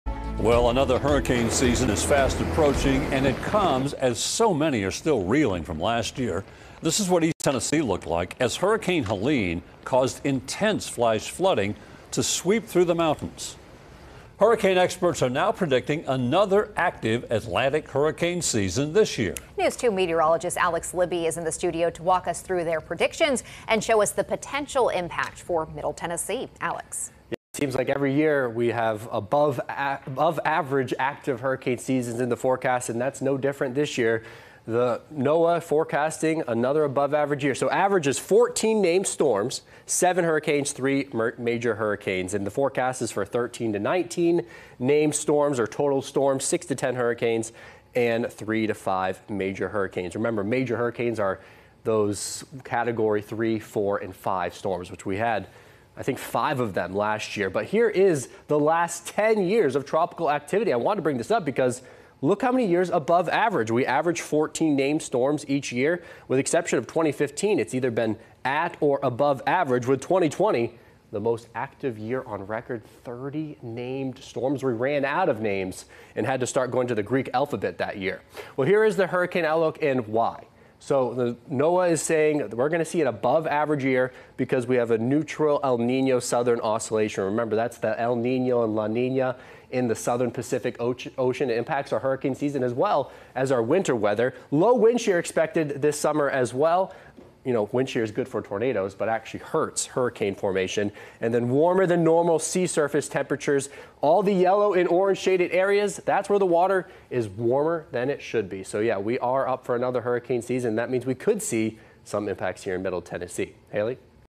Taking a look at the 2025 Atlantic hurricane season
News 2 is taking a look at the National Oceanic and Atmospheric Administration's predictions and what they could mean for Middle Tennessee.